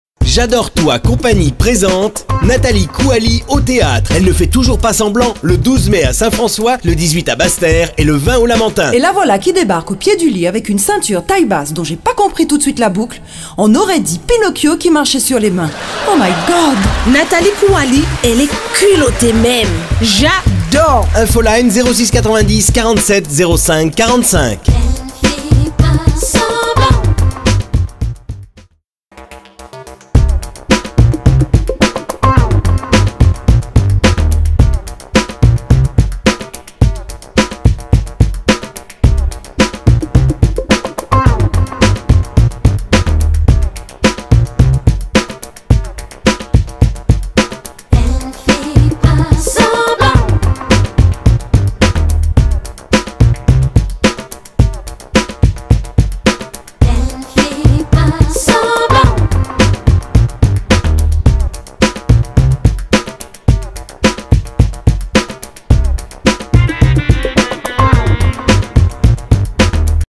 Nathaly Coualy- répétition
Un spectacle écrit et interprété seule en scène par Nathaly Coualy, co-écrit avec Bertrand Dicale et la participation de Pascal Légitimus.
Spot: Vogue Studio
Production: Jadoretoi Compagnie, Jessica Adriaaansen, Alexandra Chapey
En représentation à la Guadeloupe les 12 Mai à St François, le 18 à Basse-Terre, le 20 au Ciné-Théâtre du Lamentin.
Info et réas sur place ou au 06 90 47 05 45